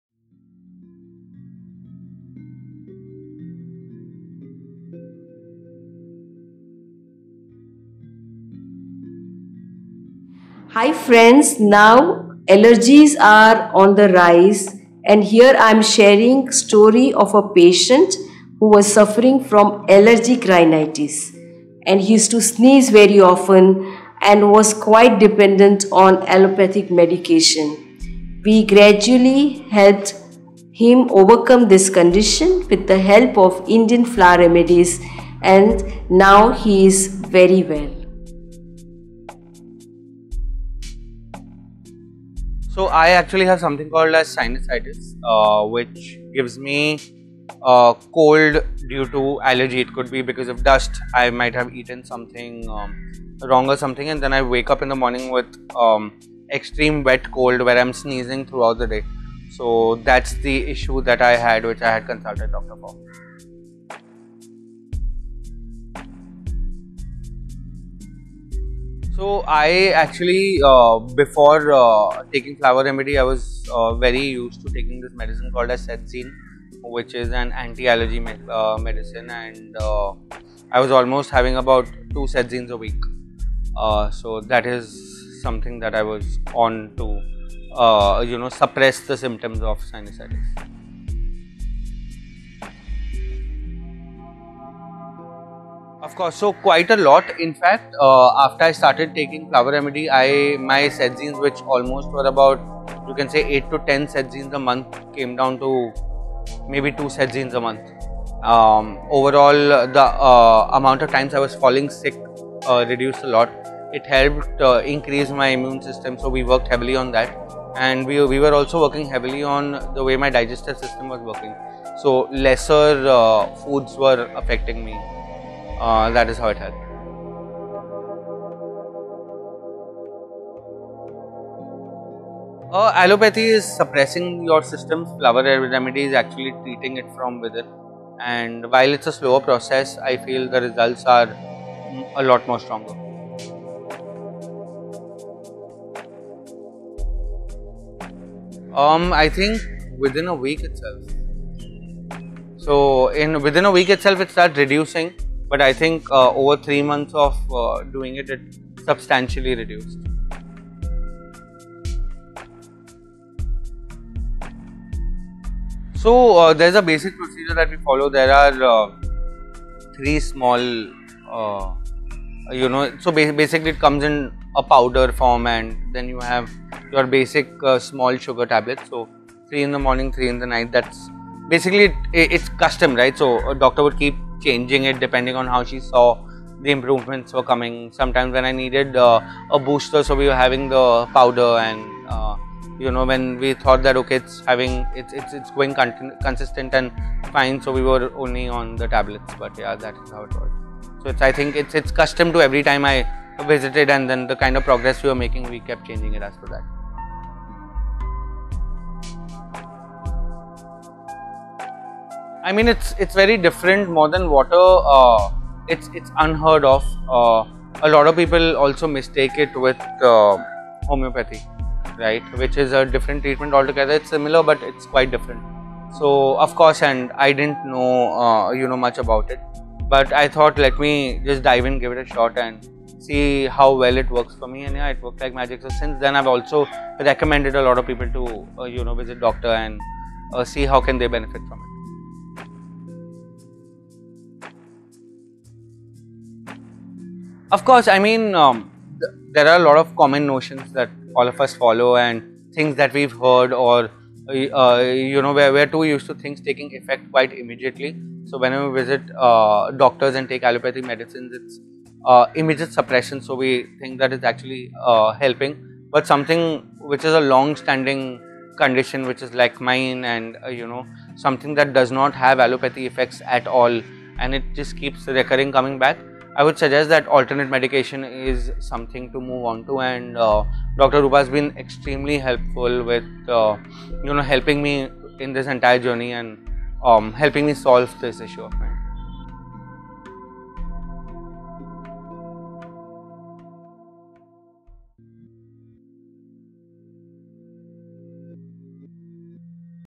Sinusitis Support Treatment By Indian Flower Remedies | Healin Temple | Dr Rupa Shah | Dr Atul Shah
Allergies are on the rise, story of a patient who was suffering from allergic rhinitis. He used to squeeze very often and was quite dependent on allopathy medication. We gradually helped him overcome the condition with the help of Indian Flower Remedies. Now he's doing good.

Coming up next workshop Flower Power Level 2 in Mumbai on 4/5 February 2023.